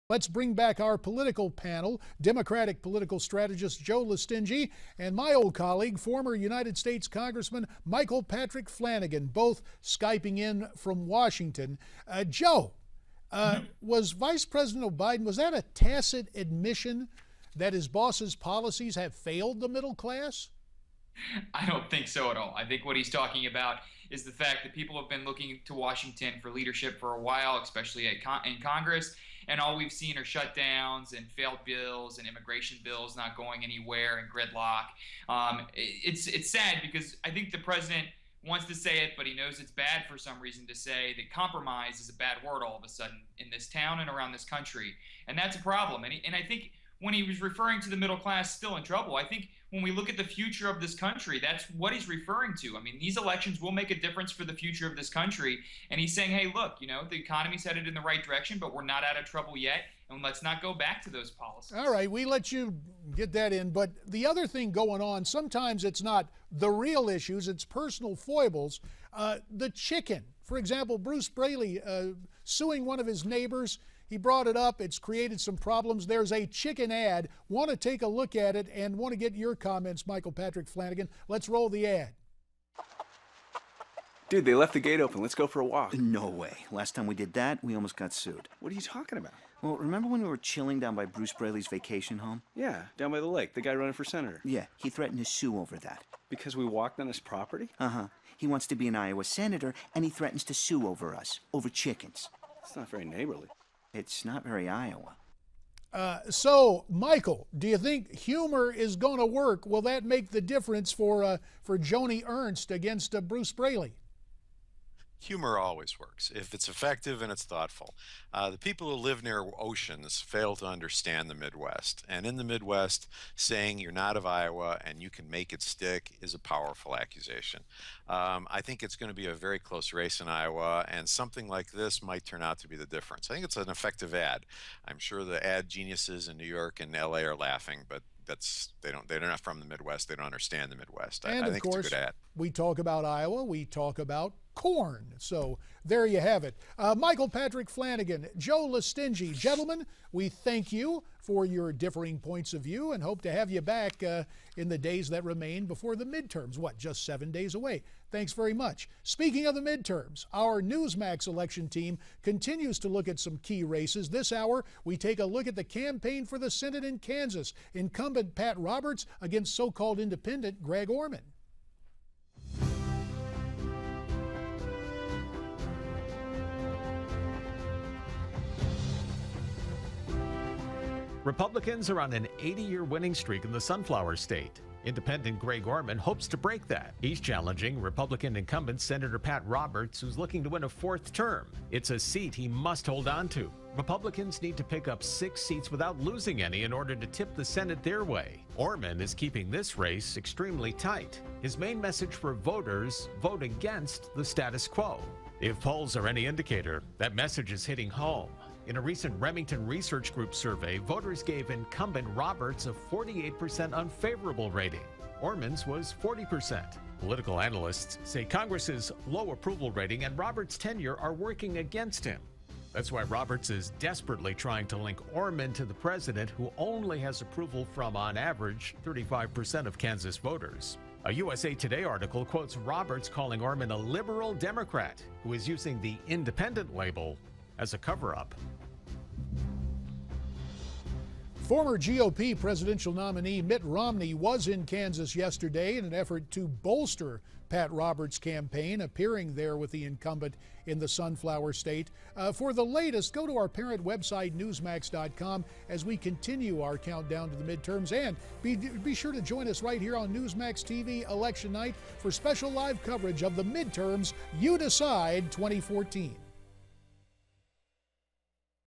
Iowa Chicken Ad Aims to Play on Humor
Joe Lestingi and Michael Patrick Flanagan discuss the Iowa Senate race.